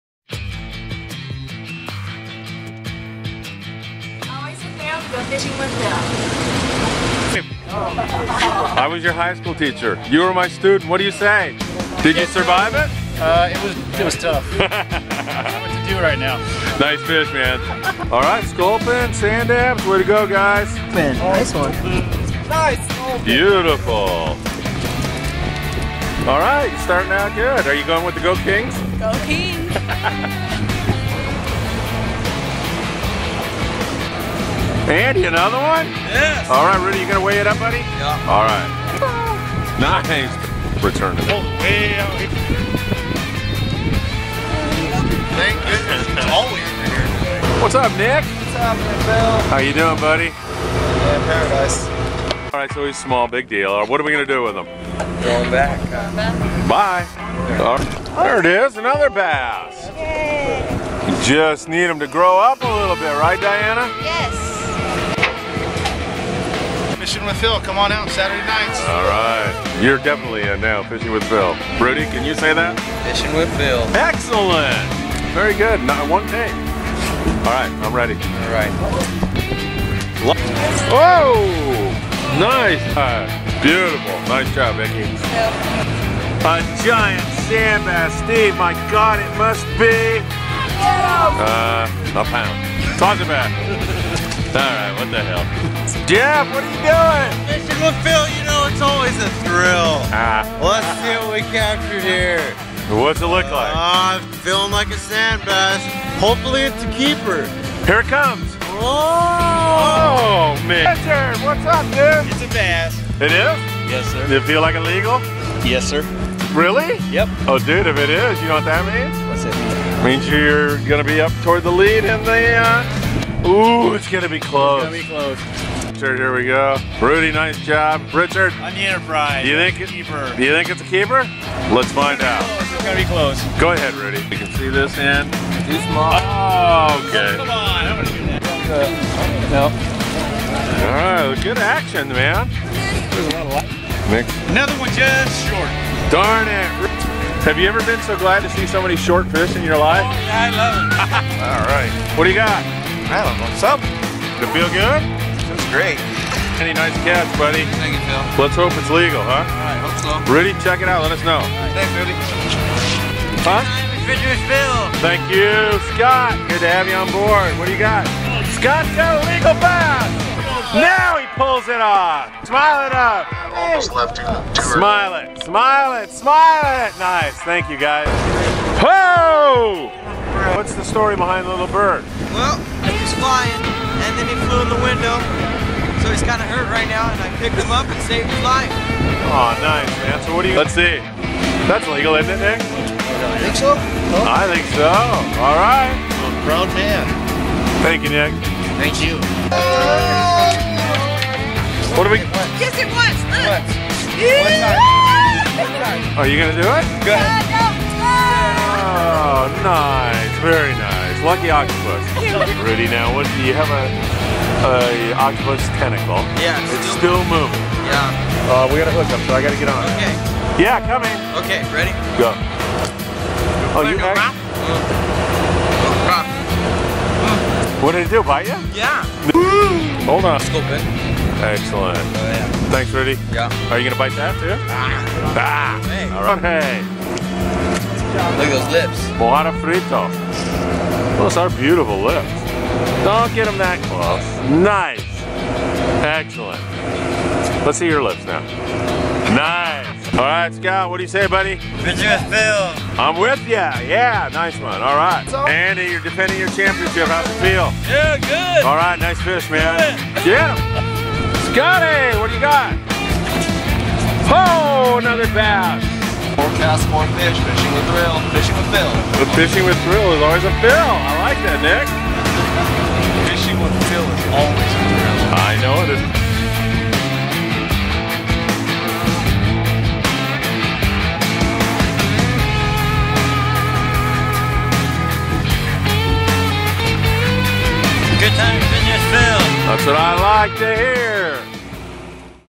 Big Bad Bass & Barracuda Bash First Weekend with Phil
The first weekend of the Phil Friedman Outdoors Big Bass & Barracuda Bash saw very slow fishing as we fought a wicked downhill current. That current is gone and things are looking up for the second of three consecutive Friday and Saturday nights.

Just book with Phil to go Twilight Fishing this Friday and Saturday for FREE ENTRY into the Bash as well as a 1/2 pound bonus each time you sign up.

See you this weekend. ONLY $40. Text/Call Phil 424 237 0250